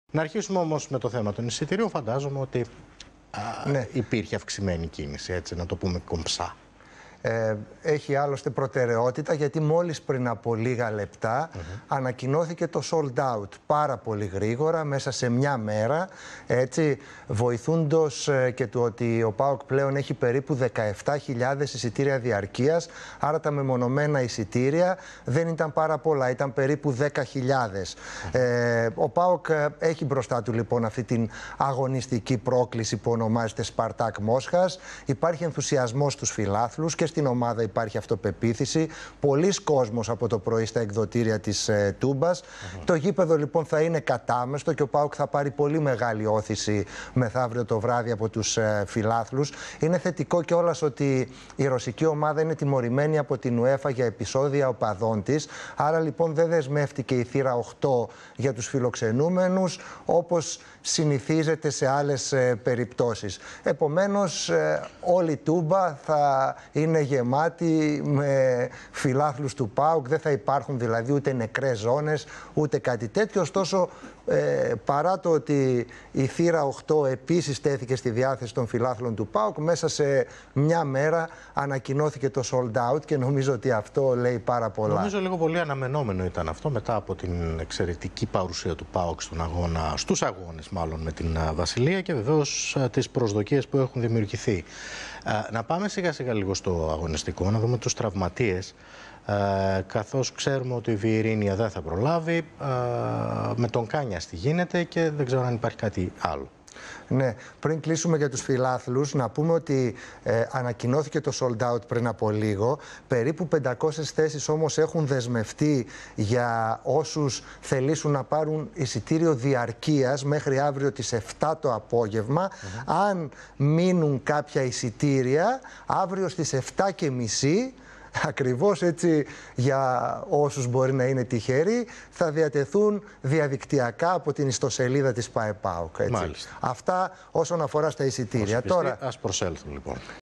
Champions League: Εξαντλήθηκαν τα εισιτήρια για τον αγώνα ΠΑΟΚ - Σπαρτάκ Μόσχας!
Με ανακοίνωση της η ΠΑΕ ΠΑΟΚ σαράντα οχτώ ώρες πριν από το πρώτο ματς με την Σπαρτάκ, με το οποίο γνωστοποιείται το τέλος των εισιτηρίων για την αναμέτρηση της Τετάρτης (8/8).

Ο κόσμος του ΠΑΟΚ «εξαφάνισε» τα εισιτήρια του πρώτου αγώνα με την Σπαρτάκ Μόσχας υποχρεώνοντας την «ασπρόμαυρη» ΠΑΕ να ανακοινώσει το απόγευμα της Δευτέρας (6/8) sold out.